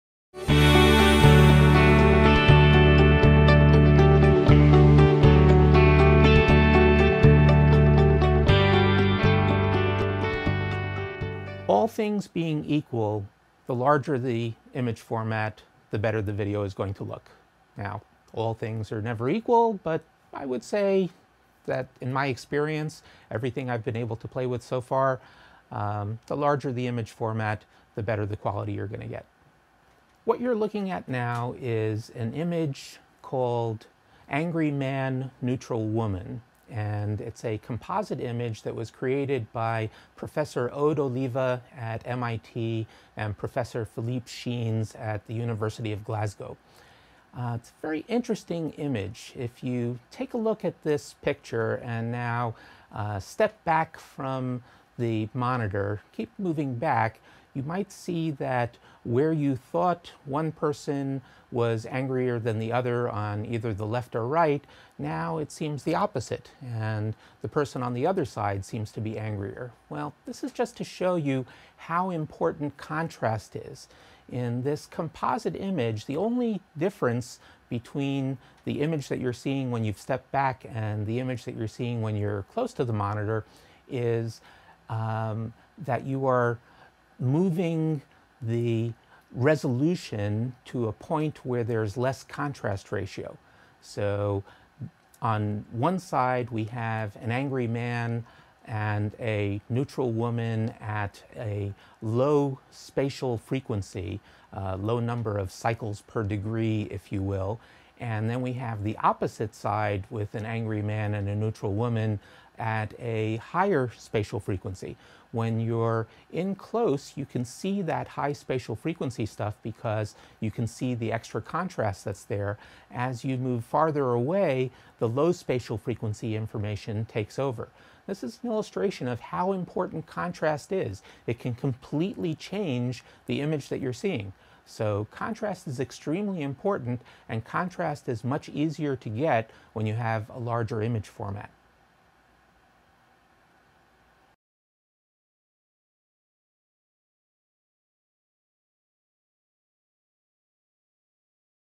Camera Imager Size and Quality - An interview with Mark Schubin
HD Camera Guide interviews technology consultant Mark Schubin about the quality expectations in HD video based on imager format size.